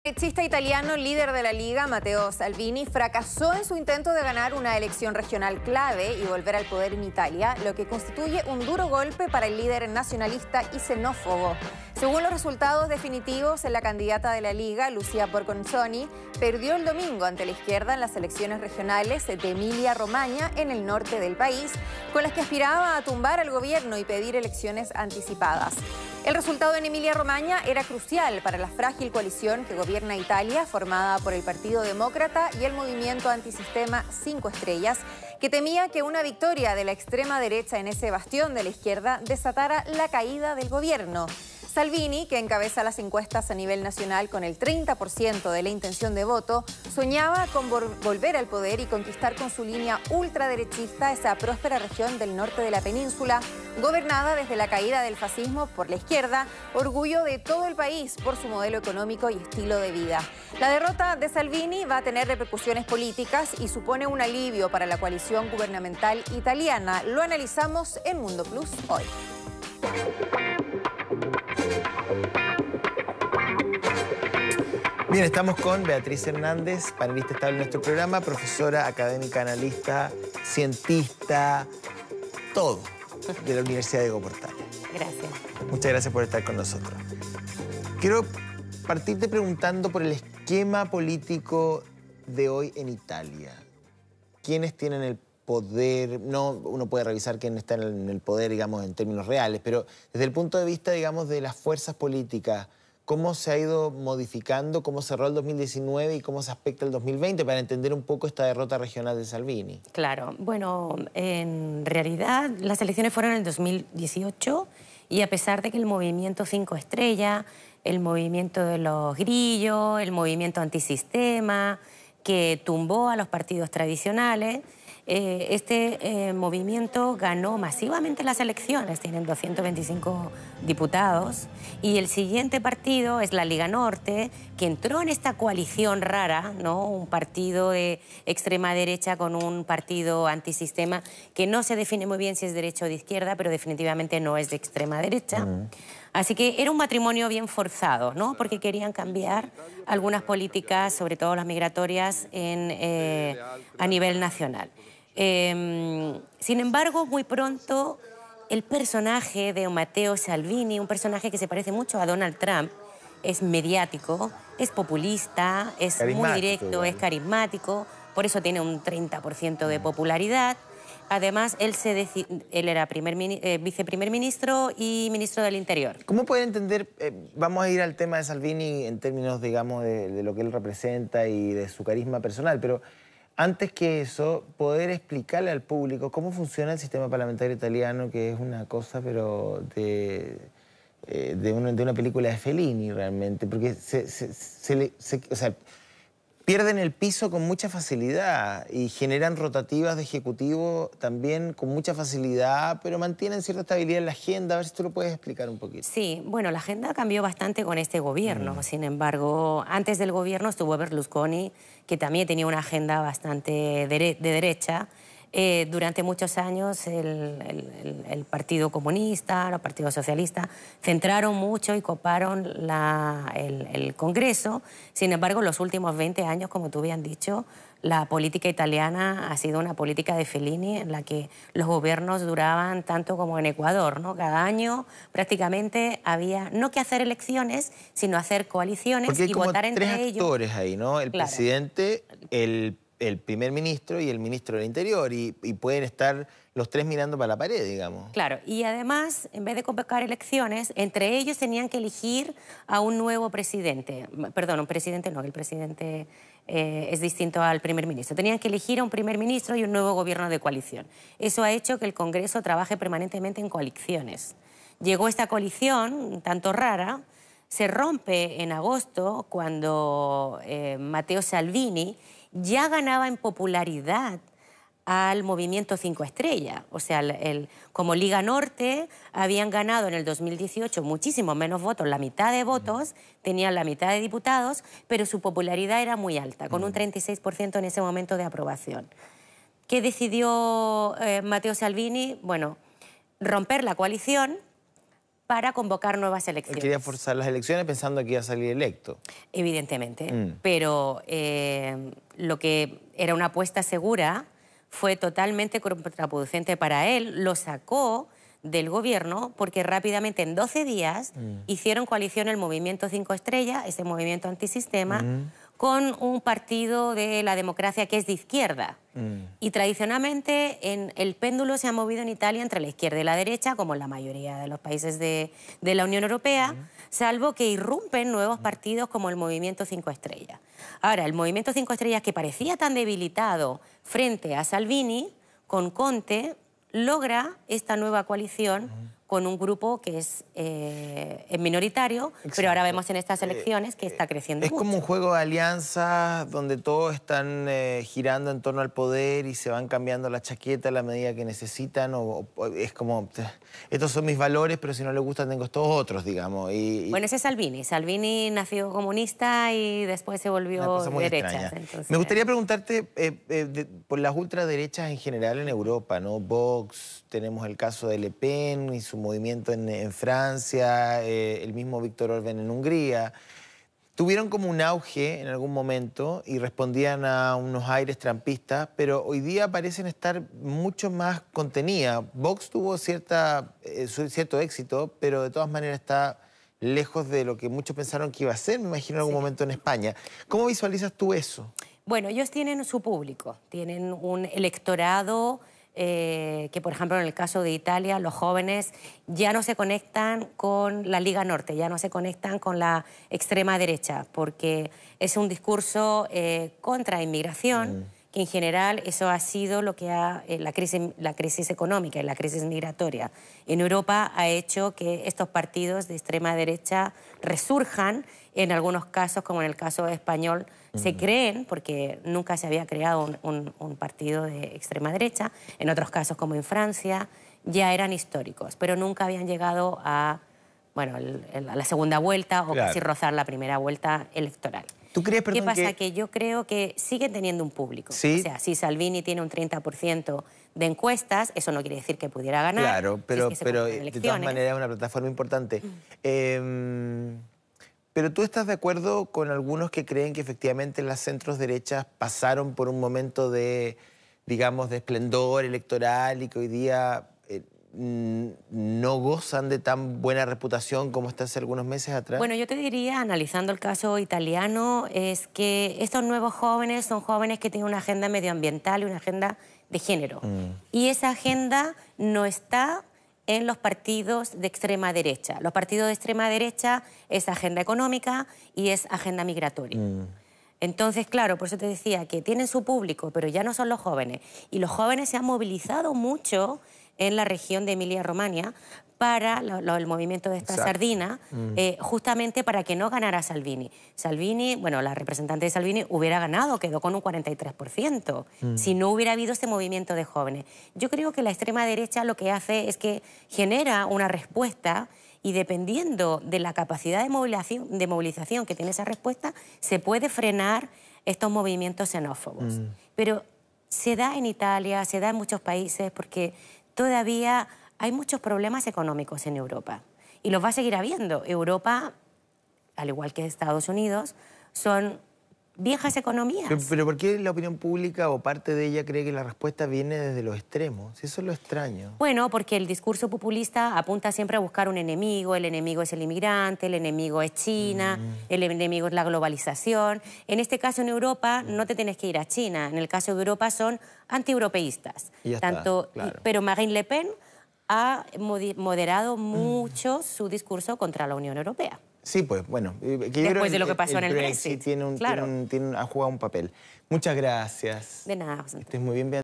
Matteo Salvini fracasó en su intento por forzar elecciones generales anticipadas
El ultraderechista italiano Matteo Salvini, sufrió una dura derrota en las elecciones regionales de Emilia Romagna en el norte de Italia, su objetivo de forzar elecciones generales anticipadas se aleja por el momento.